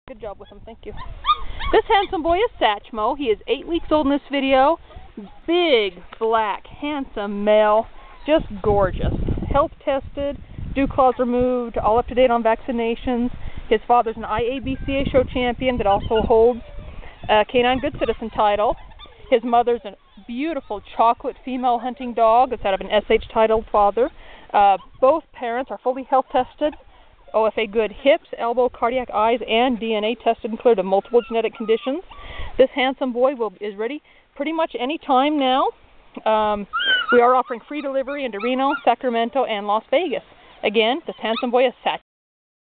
Satchmo at just over 8 weeks old
Satchmo huge black male ready now for his lucky new home, ch. sired health tested parents